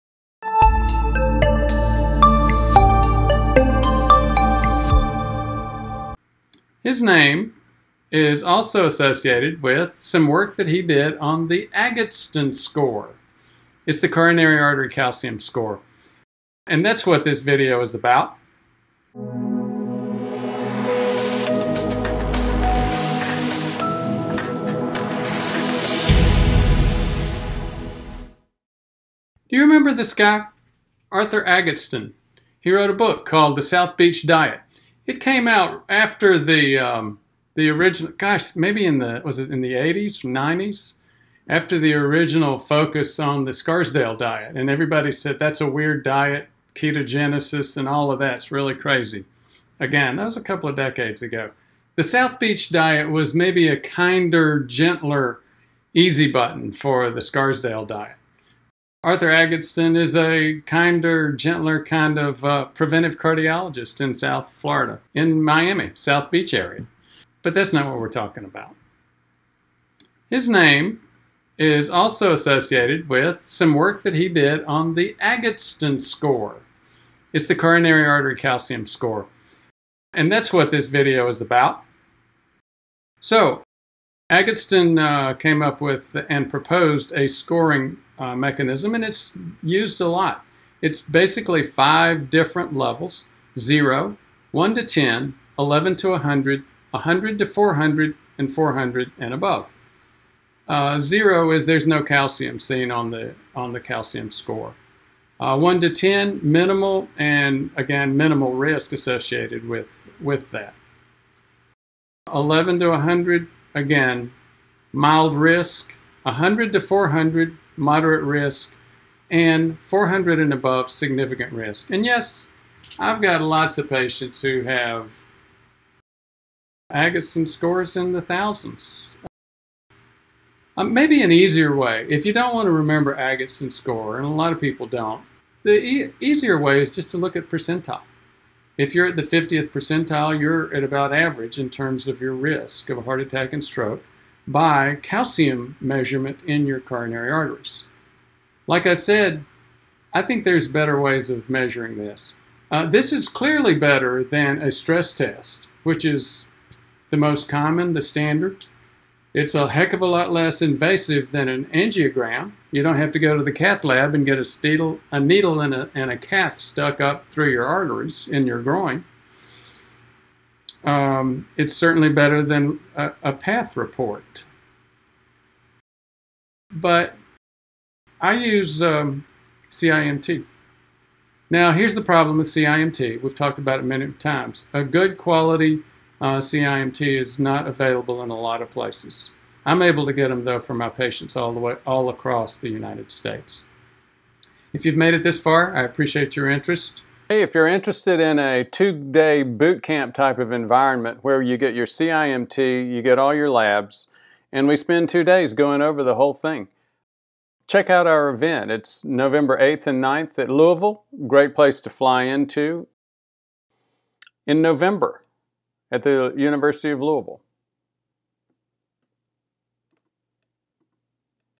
Predict Heart Attack Risk with Agatston Score
ABOUT THIS VIDEO: I give a brief description of the Agatston score (also called calcium score), a range of values used to predict heart attack risk. It is calculated based on plaque detected in a CT scan. Age and gender are factors.

Online courses (CVI Inflammation, Insulin Resistance, Plaque Evaluation, and Heart Attack & Stroke Prevention Courses)